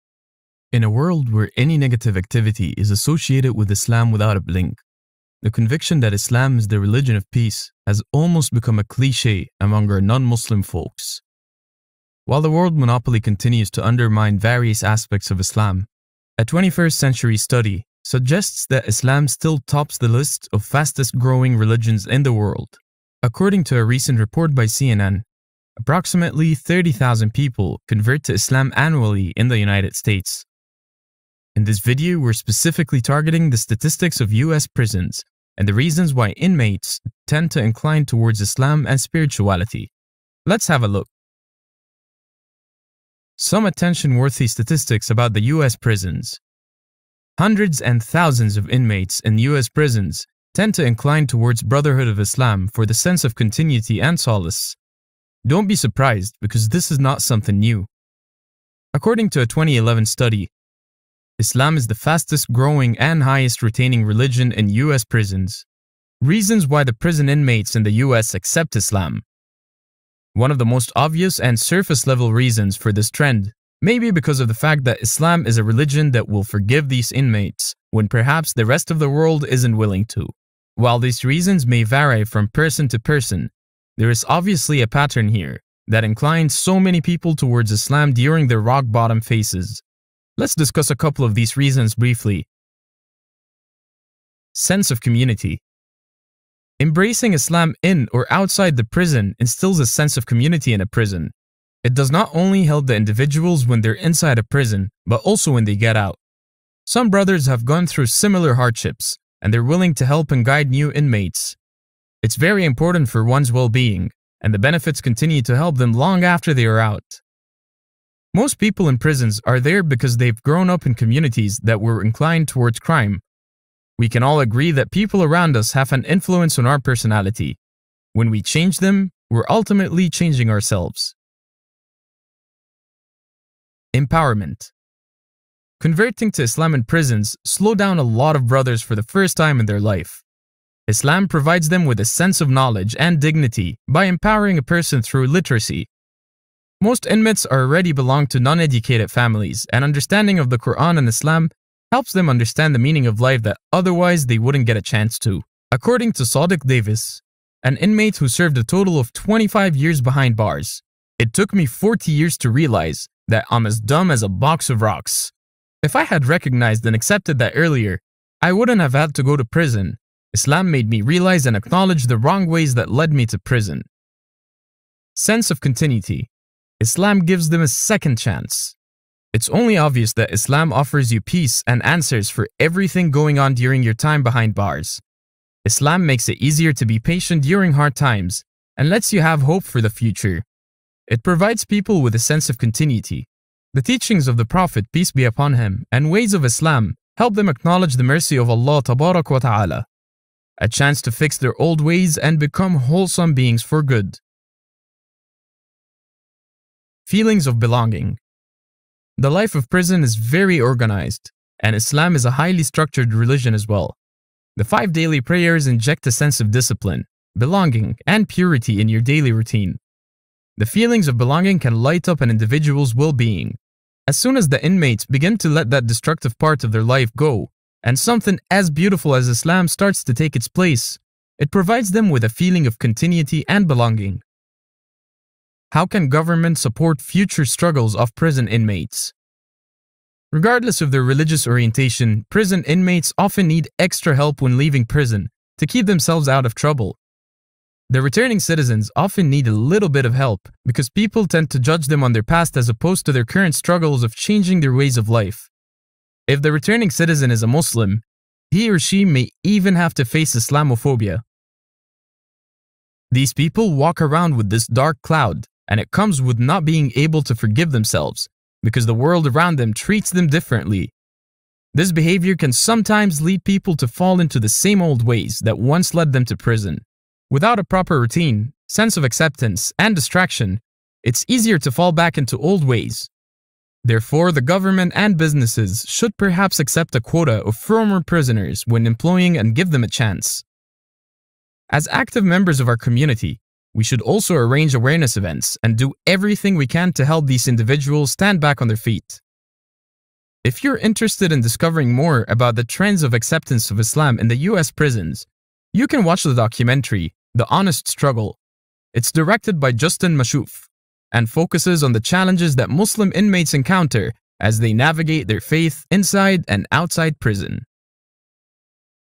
Why do so many US Prisoners Convert to Islam?
According to a recent report by CNN, approximately 30,000 people convert to Islam annually in the United States.  In this video, we’re specifically targeting the statistics of US prisons and the reasons why the inmates tend to incline towards Islam. Let’s have a look.